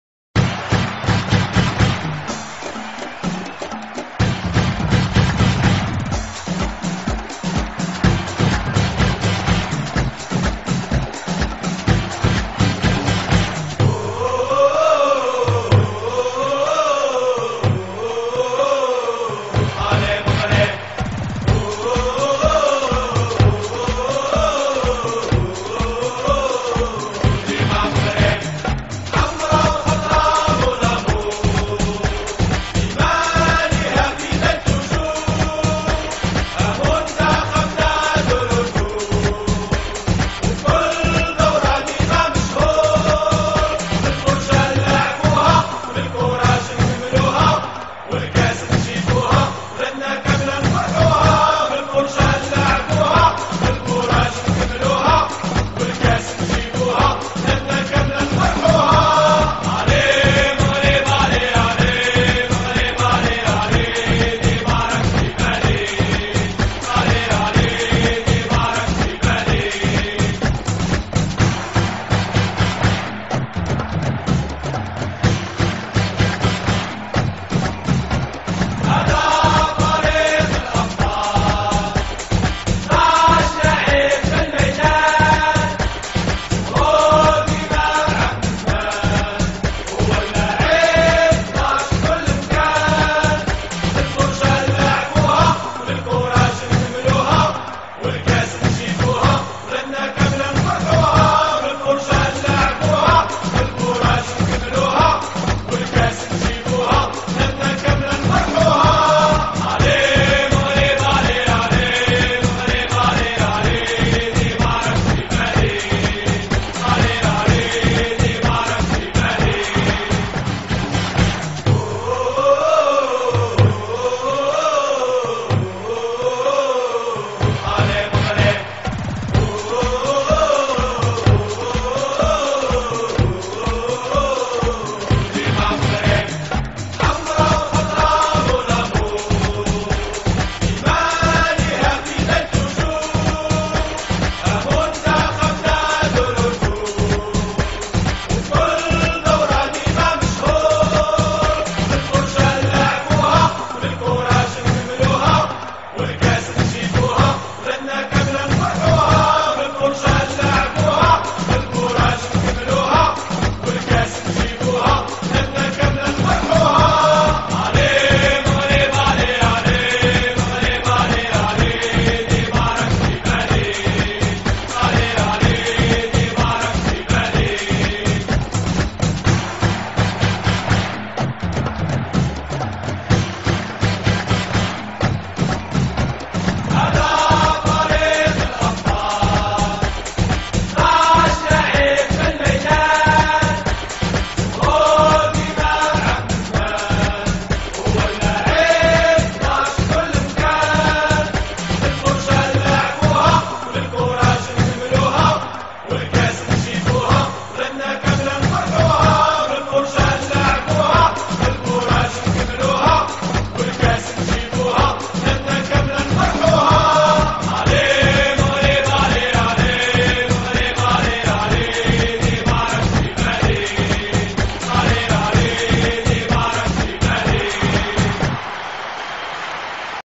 Algerie le pire match de tous les temps vous attend 4-6-2011 Marrakech Maroc 5-0 Algerie Inchaallah
L'arbitre du match Algérie-Maroc a été payé généreusement pour faire gagner l'algérie; Mais attention, le Maroc n'attend que le retour; La pression sera sur l'algérie ;on va gagne inchaallah 5 - 0 à Marrakech